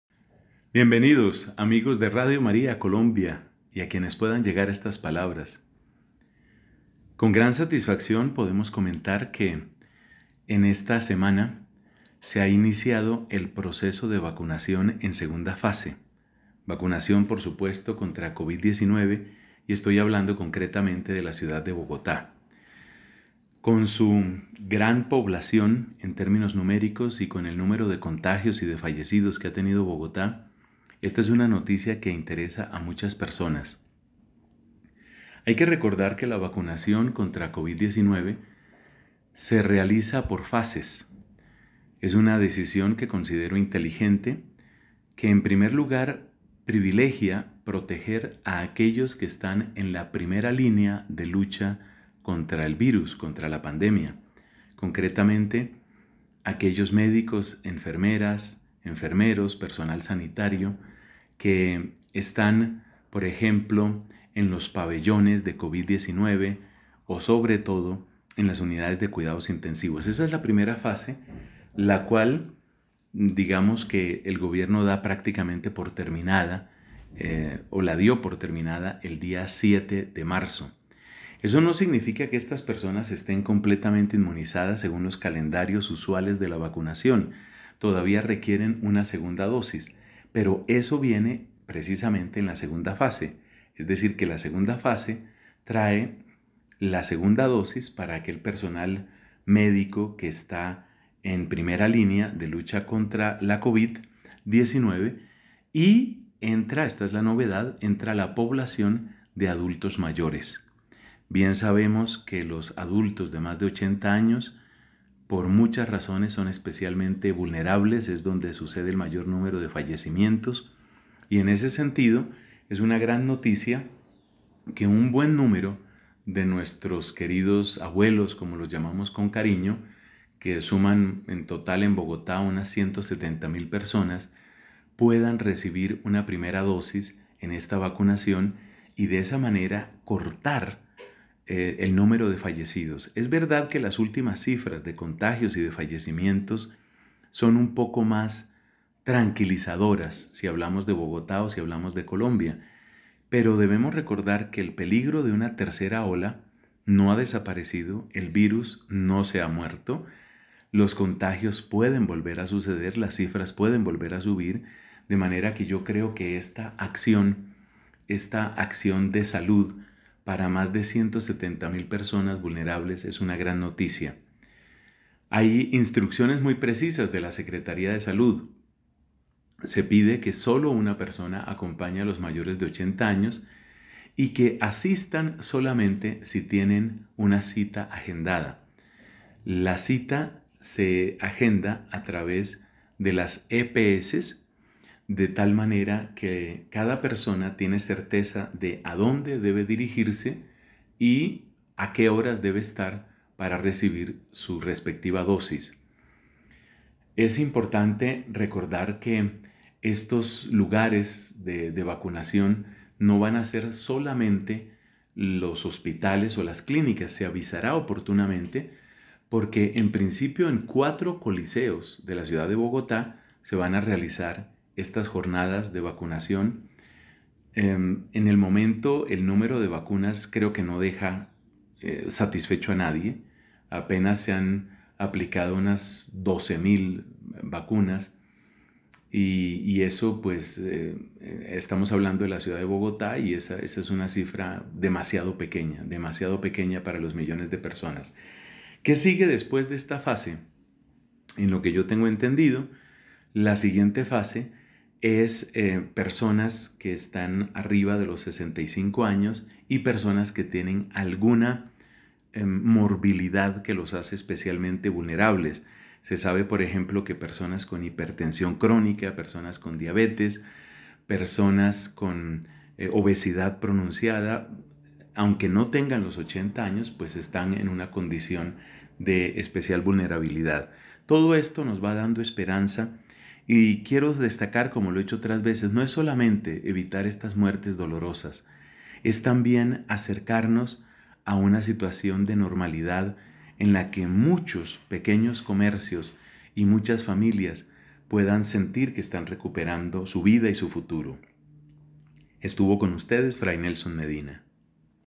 (132) Segunda fase de vacunación en Bogotá - Noticia leída desde la fe
Esta semana se inicia el proceso para vacunar a todos los mayores de 80 años de la ciudad, por lo que este fin de semana las EPS están llamando a estas personas, para asignarles un día, lugar y hora, para que acudan a recibir su primera dosis de vacuna contra el COVID-19. - Comentario de Fr. Nelson Medina, OP